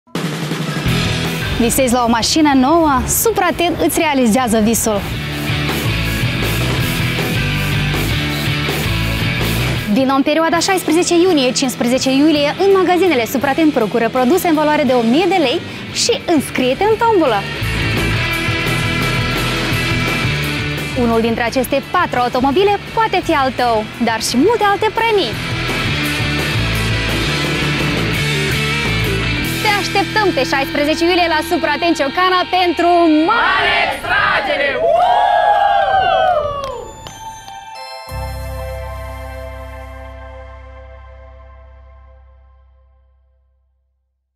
Câştigă un automobil cu SUPRATEN!
SUPRATEN îți realizează visul. Vino în perioada 16 iunie – 15 iulie 2017 la magazinele “SUPRATEN”, procură produse în sumă de minim 1000 Lei și înscrie-te la tombolă. Încearcă-ți norocul, câștigă unul dintre cele 4 automobile și alte premii!

SUPRATEN – oferte avantajoase în fiecare zi!